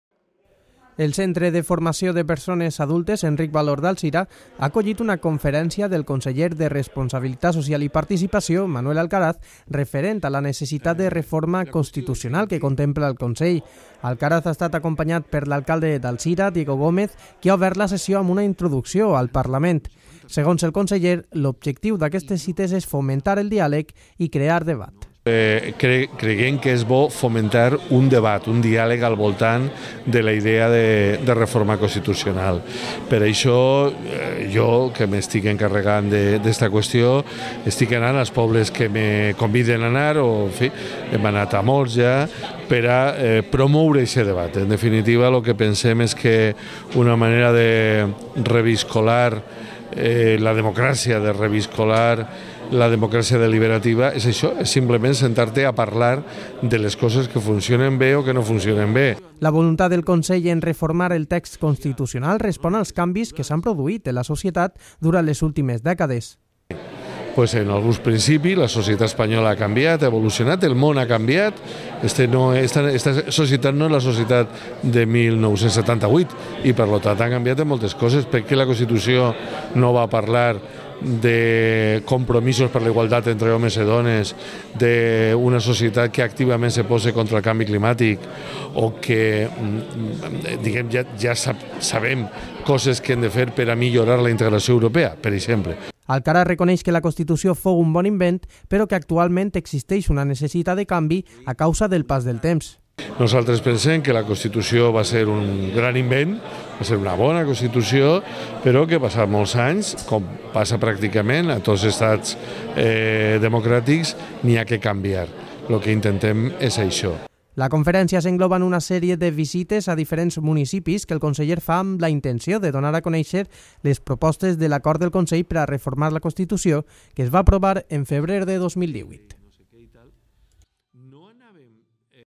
Alcaraz parla en Alzira de la necessitat de reformar la Constitució
El Centre de Formació de Persones Adules Enric Valor d’Alzira ha acollit una conferència a càrrec de Manuel Alcaraz, conseller de Transparència, Responsabilitat Social, Participació i Cooperació, sobre les reformes del text constitucional que proposa el Consell.